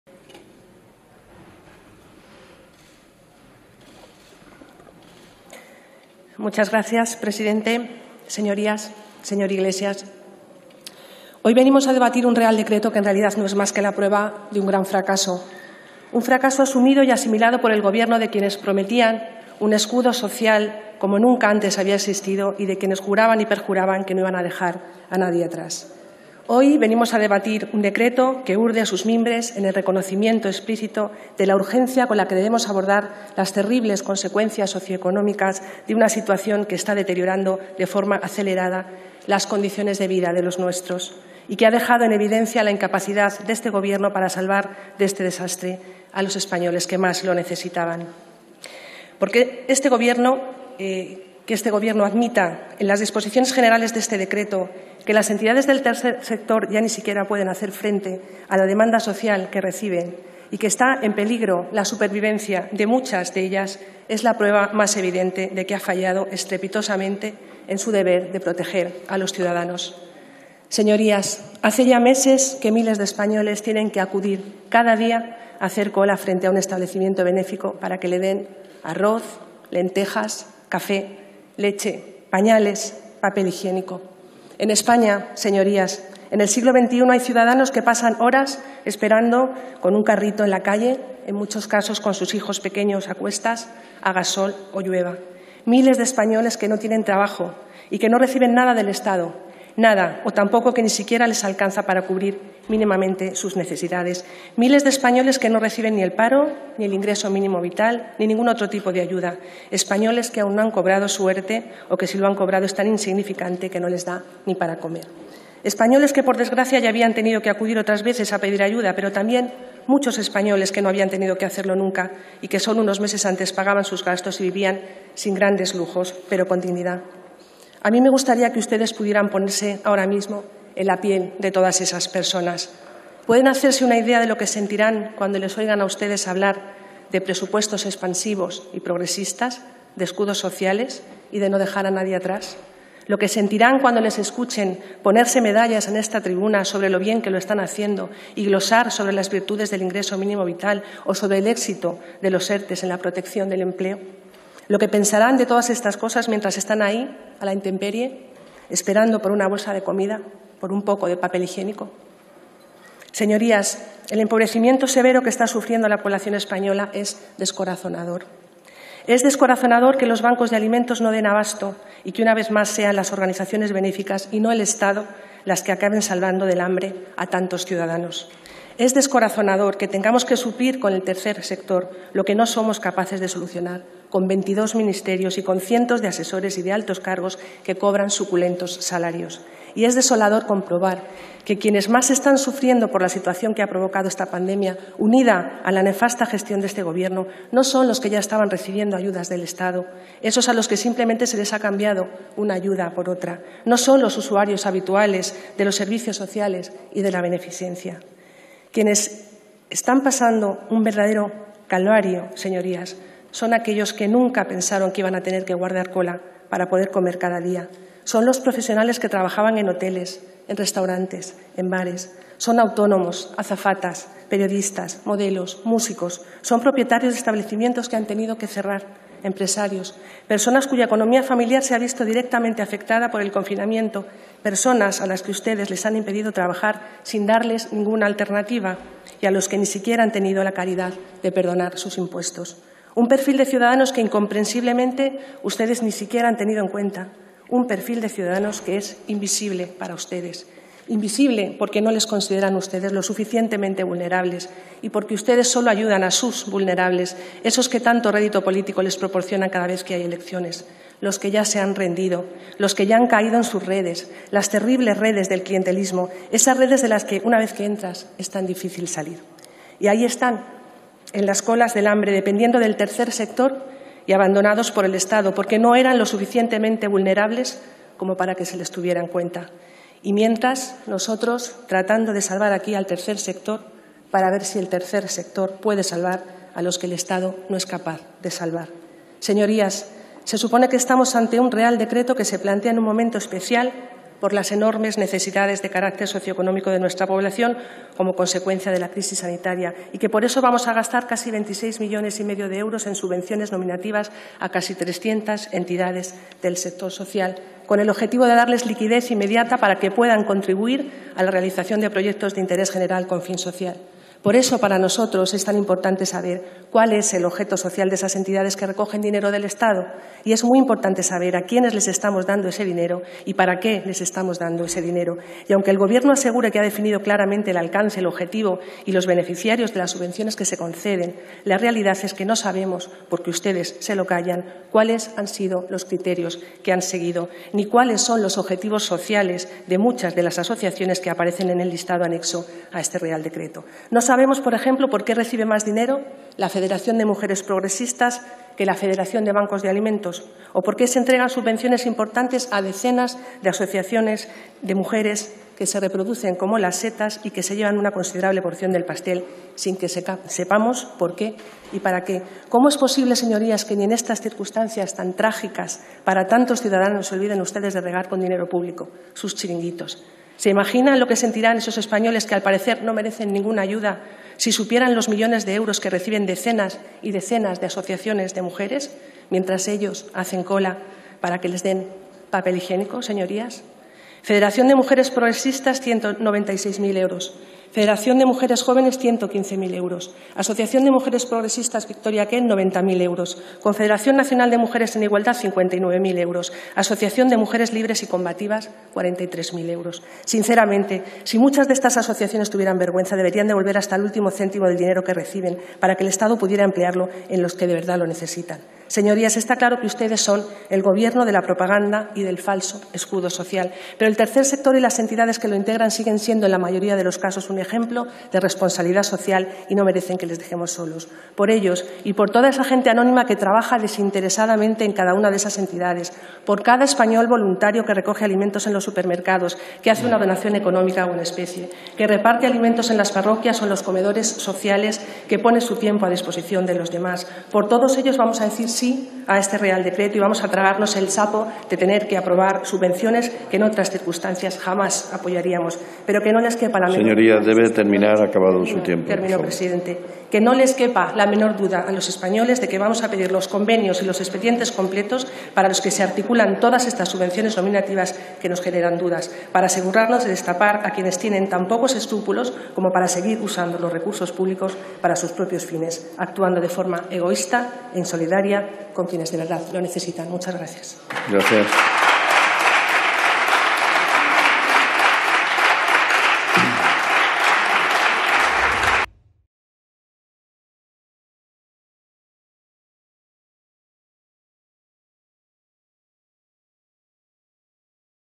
María Ruiz: “¿Por qué reciben más dinero las mujeres progresistas que el banco de alimentos?”
La portavoz de VOX en la Comisión de Trabajo, Inclusión, Seguridad Social y Migraciones, califa de “fracaso” la gestión del Gobierno “que prometía un escudo social y ha fallado estrepitosamente en su deber de proteger a los ciudadanos”. Además acusa al Ejecutivo de regar con dinero a sus chiringuitos feministas en mitad de la crisis.